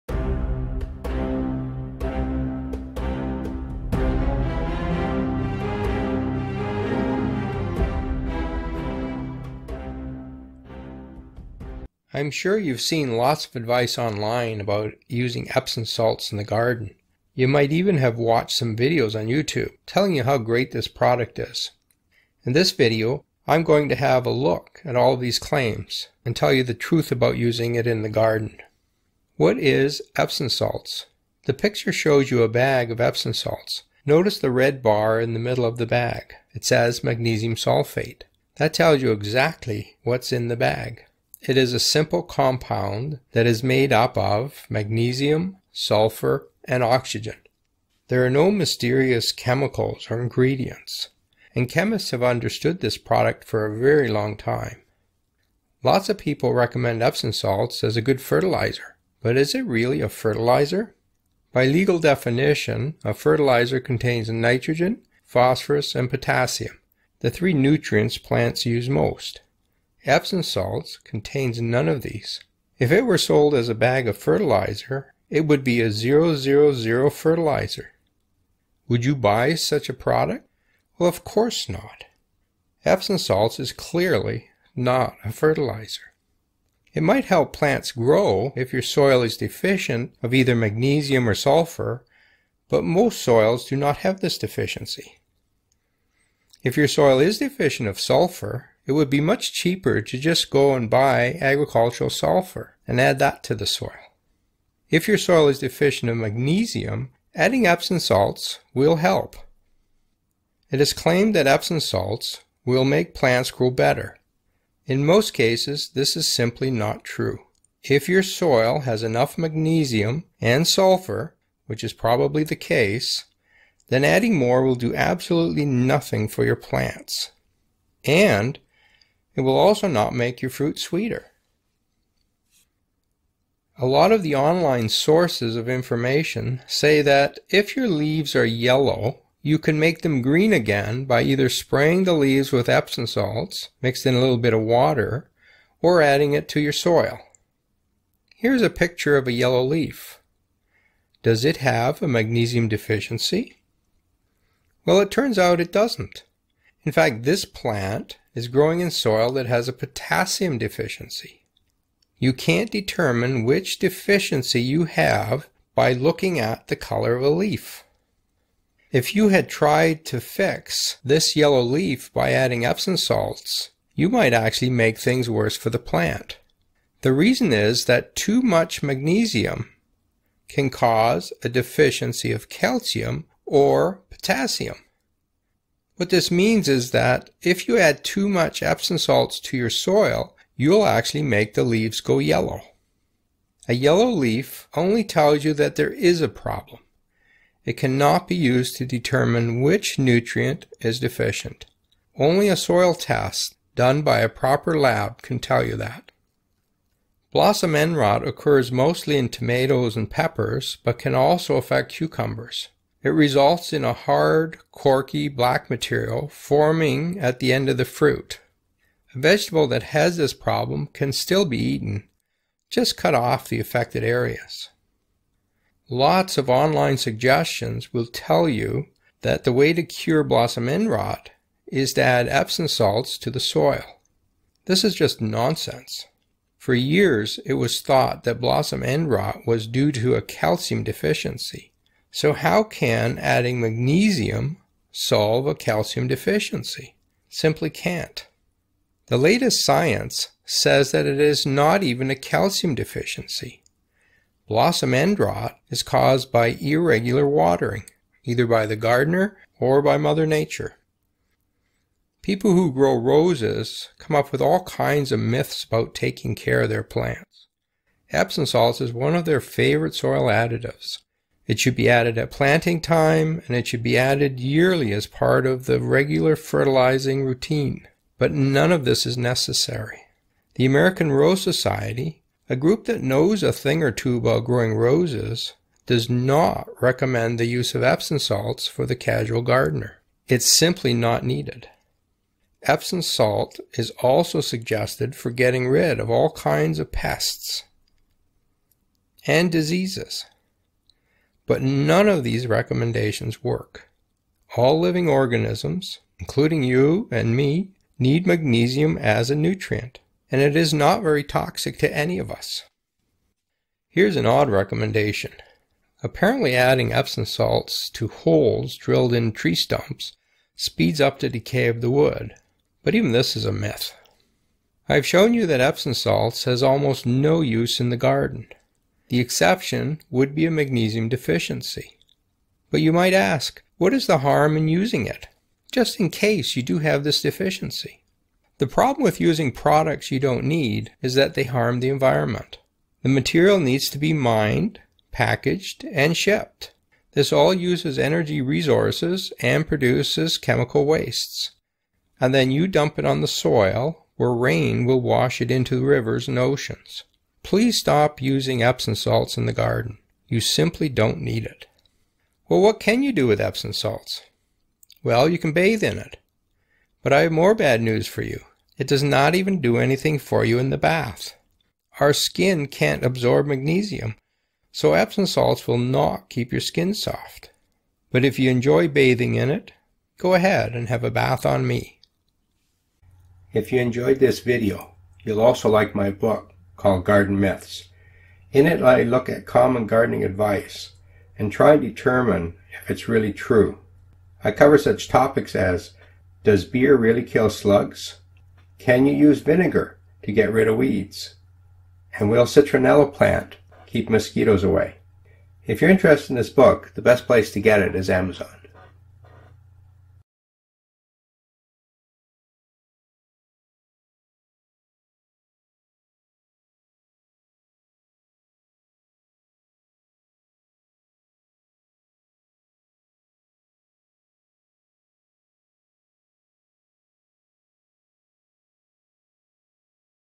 Epsom Salt Myths - learn the truth about using it in the garden [new research]
Find out the truth about Epsom salts in the garden. This video reviews all of the claimed benefits of using Epsom salts in the garden and weeds out the myths.

The following claims are discussed:
- it is a great fertilizer
- it will make plants grow better
- Epsom salts deters pests and cures diseases.
- makes fruit taste sweeter
- speeds up the decay of stumps
- Epsom salts is an excellent soil additive for roses

List of Credits:

All other slides belong to GardenFundamentals.com or are public domain images.